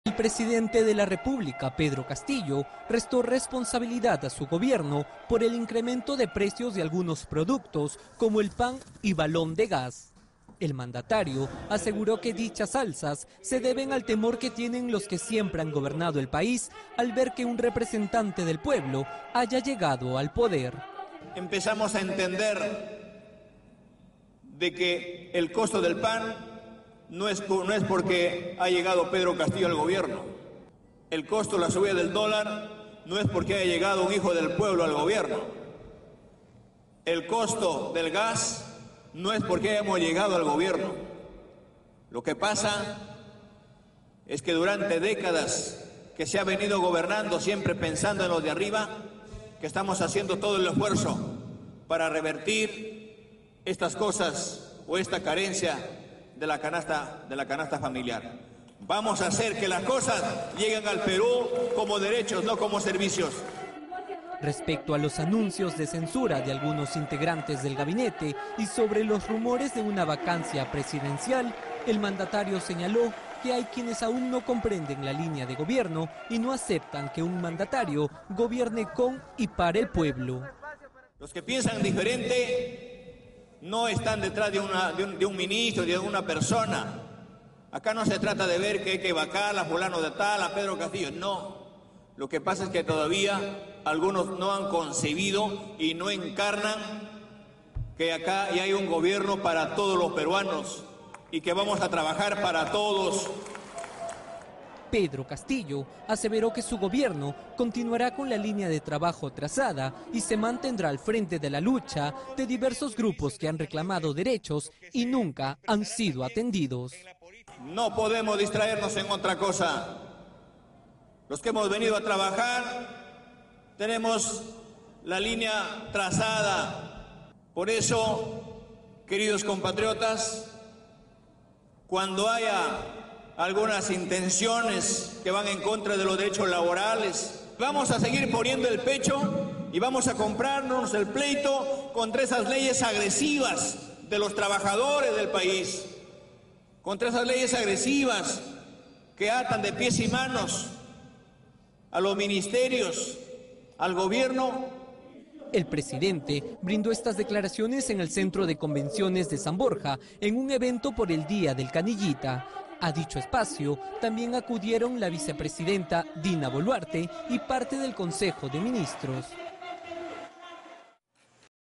Castillo sobre alza de precio pan y dólar:“No es porque haya llegado un hijo del pueblo al Gobierno”
El presidente de la República, Pedro Castillo, dijo este martes que la subida del precio del pan, del dólar o del gas no son producto de que “haya llegado un hijo del pueblo al gobierno”.

"Lo que pasa es que, durante décadas, se ha venido gobernando siempre pensado en los de arriba y tienen el temor que, en poco tiempo, demostremos que los hombres del pueblo sí podemos trabajar para el pueblo y los hombres y mujeres", aseguró el mandatario.

Asimismo, el jefe de Estado señaló, durante un evento en el Centro de Convenciones de San Borja por el 'Día del expendedor de diarios, revistas y loterías, canillita', que su gestión está "haciendo todo el esfuerzo para revertir" el incremento de costos en la canasta básica.

AFIRMA QUE LLEGÓ AL GOBIERNO A TRABAJAR

Pedro Castillo enfatizó que ha llegado al Gobierno a trabajar y no para distraerse en otros asuntos. "Por eso, cuando haya algunas intenciones que van en contra de los derechos laborales, sabemos muy claro por qué y por quiénes estamos acá", añadió.